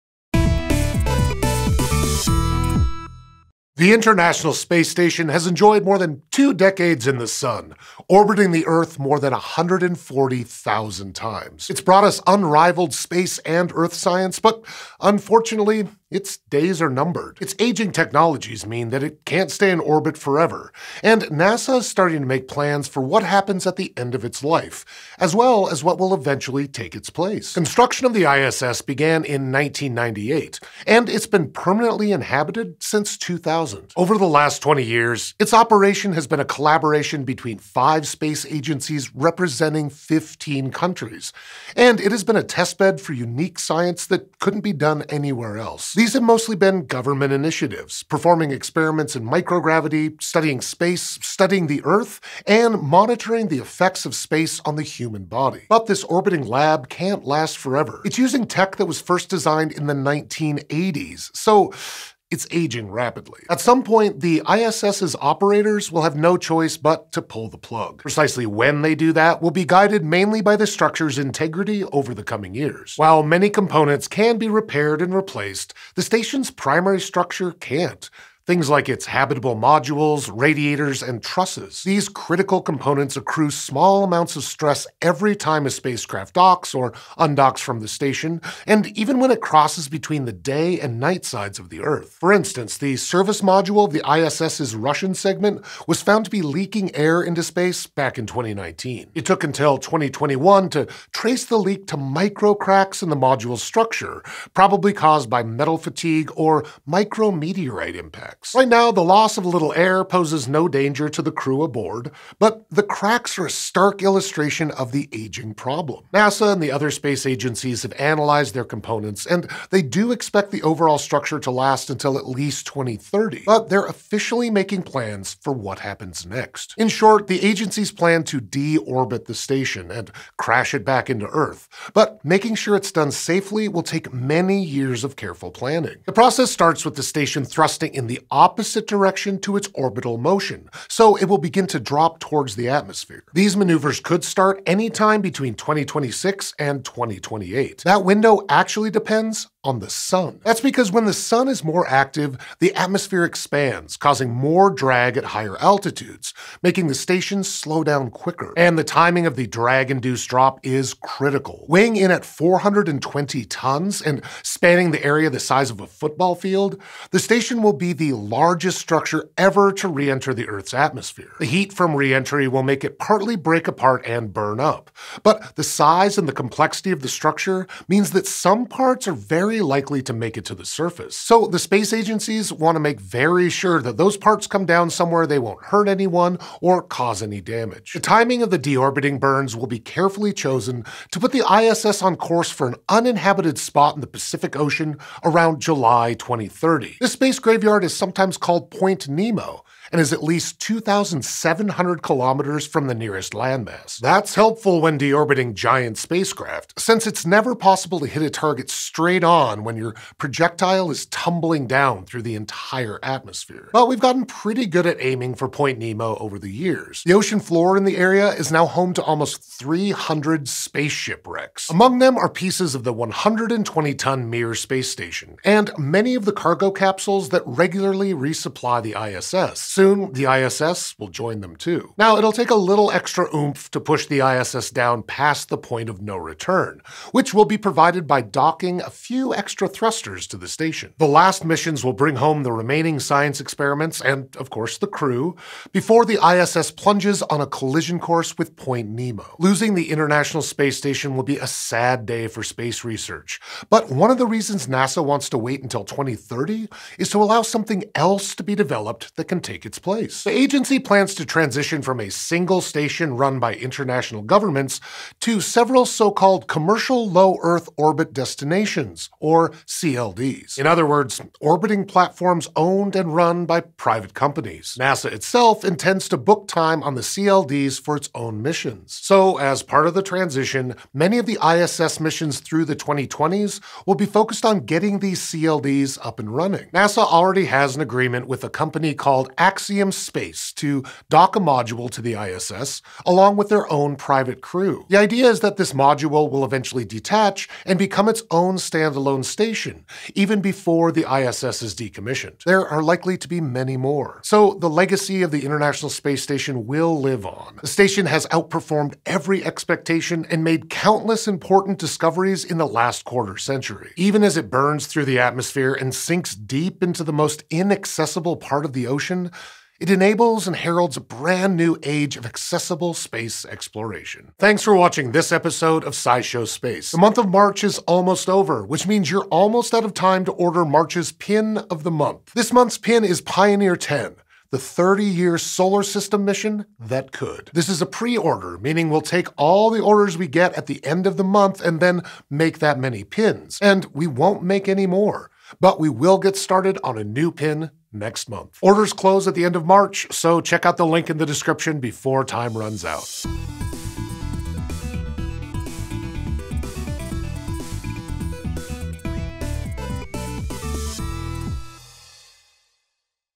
What Will Happen to The ISS?
After more than two decades buzzing around above our heads, the life of the ISS will soon be coming to a close. But what does that actually look like? And what does it mean for the future of space experimentation?

Or by checking out our awesome space pins and other products over at DFTBA Records